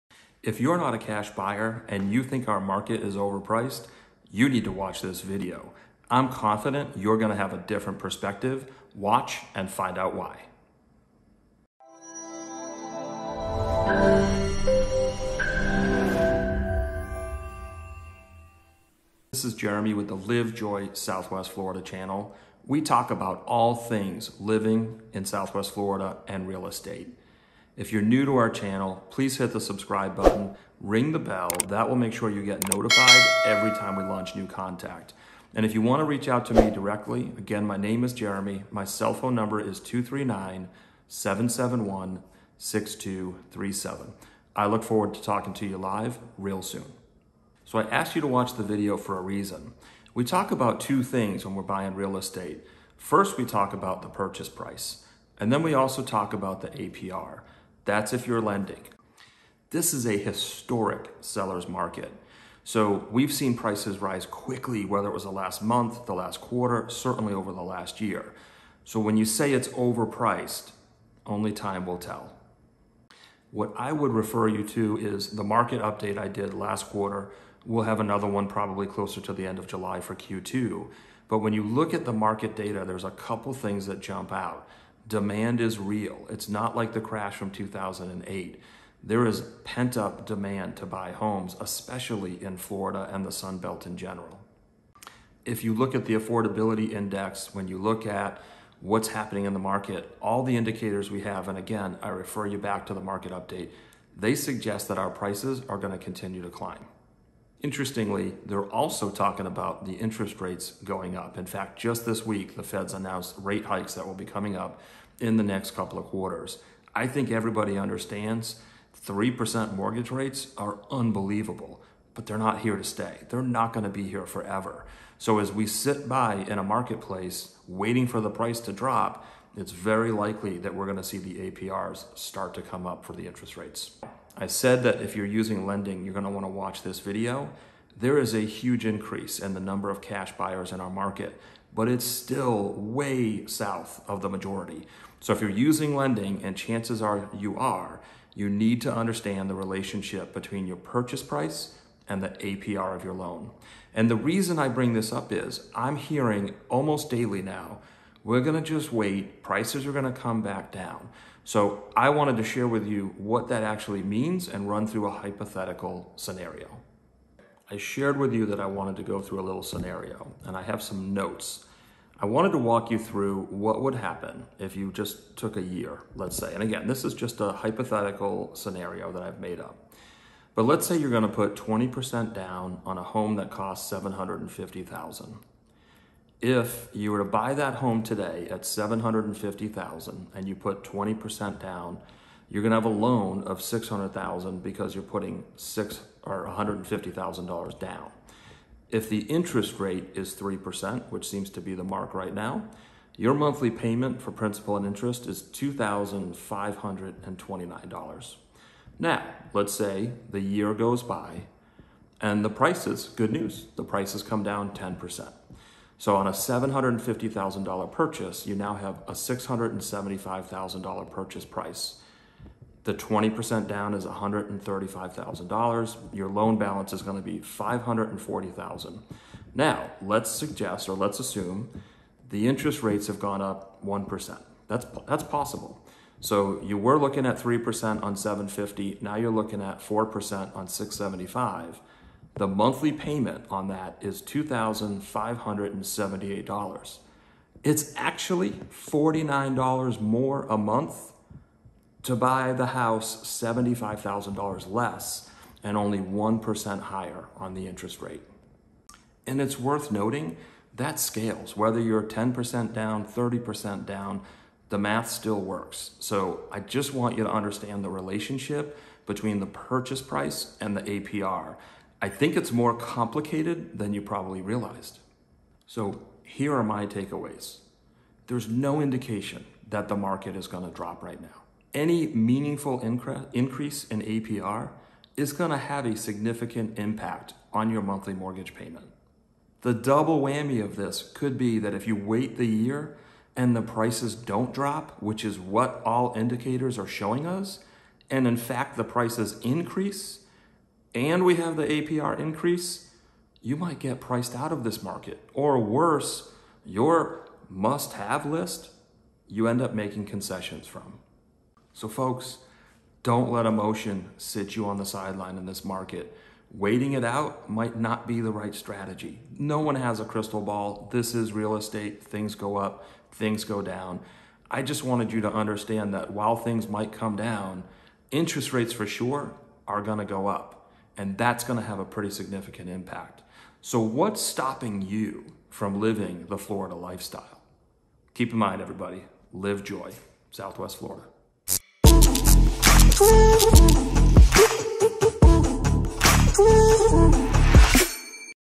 Purchase Price or APR - Which matters more?
Jeremy walks through the complicated relationship between the price you pay for a home and the APR if you are using lending.  With rising home values, many buyers feel it could be a good time to wait it out or wait for the market to drop.  With interest rates remaining at historic low levels that plan could backfire.  Watch the video to find out why!